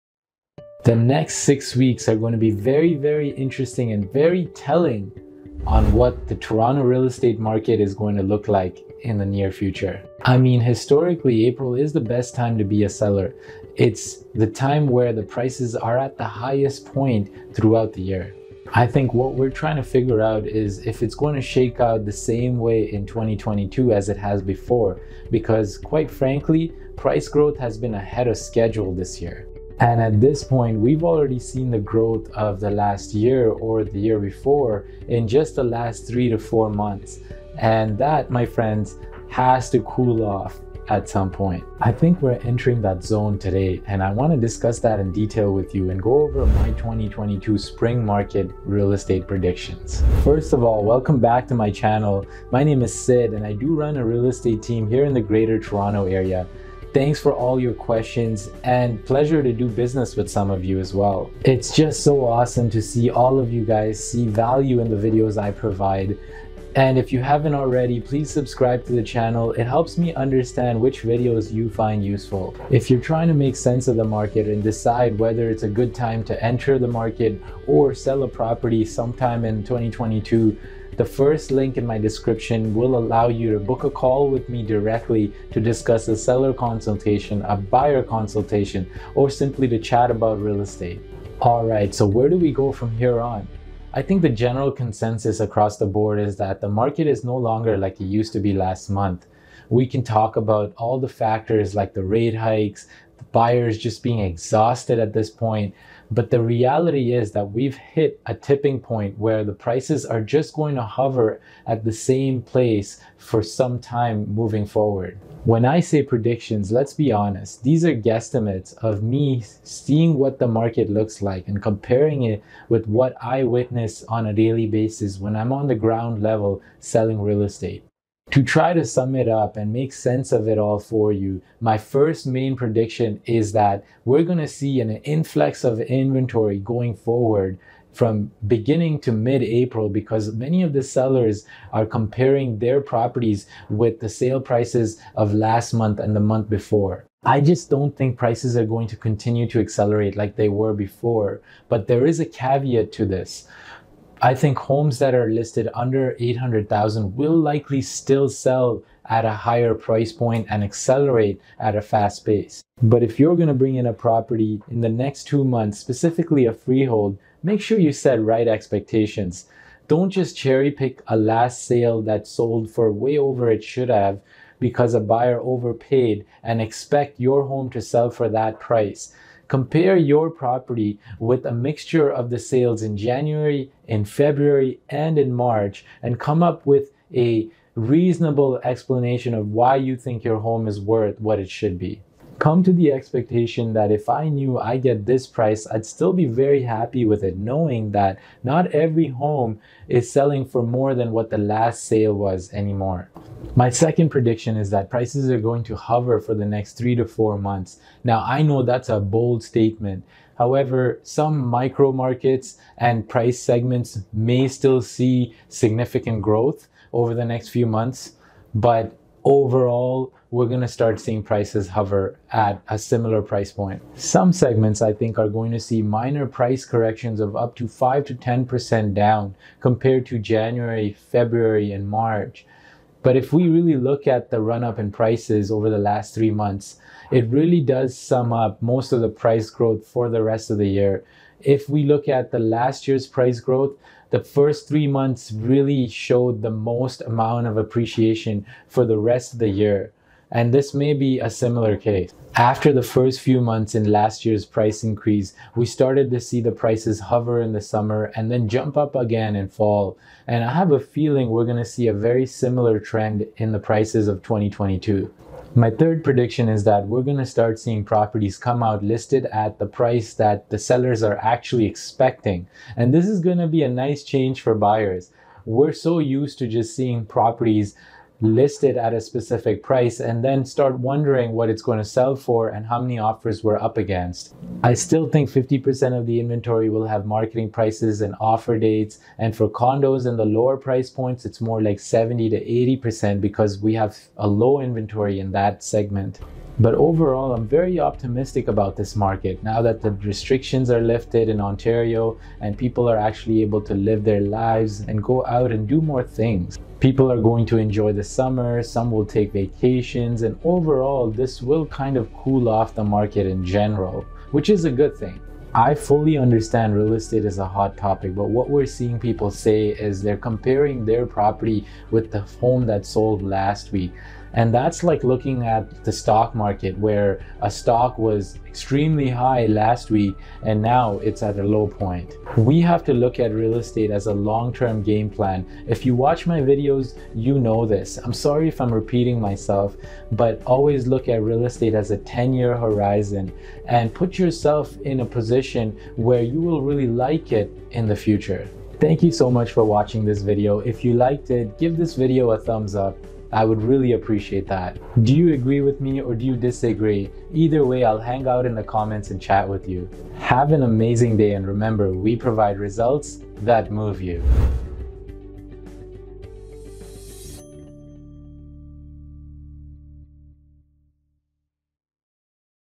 How Will The Toronto Real Estate Market Playout in Spring 2022?
In this video, I am going to talking about what we can expect in Toronto Real Estate Market.

Time Stamps:
00:00 – Intro
00:10 – Historically The Best Time To Be a Seller Is April
01:32 – The Market Today Is Not Like It Was
02:12 – 1st Prediction
03:24 – 2nd Prediction
04:25 – 3rd Prediction
05:32 – Look at Real Estate As a Long Term Game Plan
05:48 – Outro